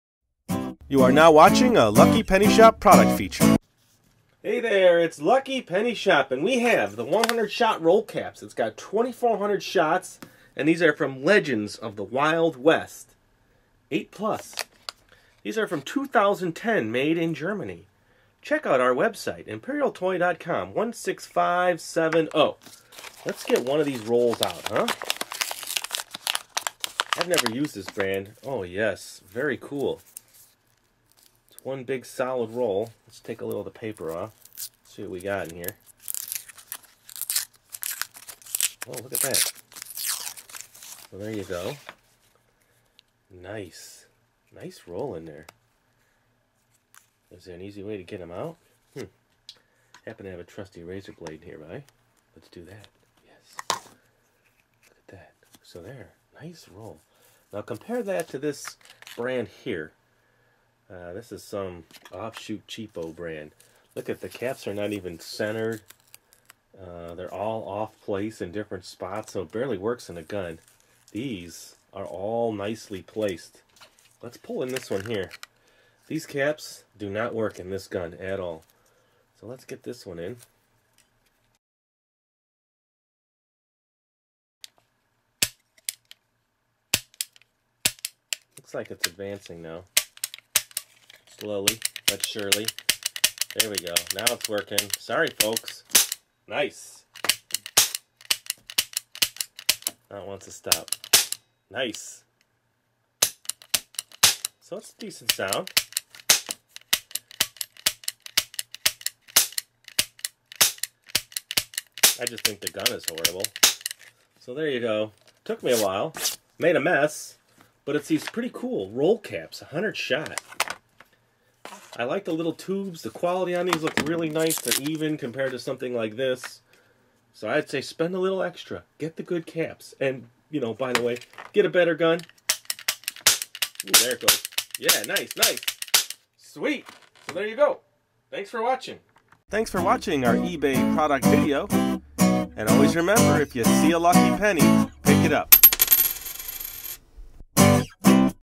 100 Shot Roll Caps - Legends of The Wild West, 2010 Imperial Toy Germany
​​​Watch our product feature video for 100 Shot Roll Caps - Legends of The Wild West, 2010 Imperial Toy Germany.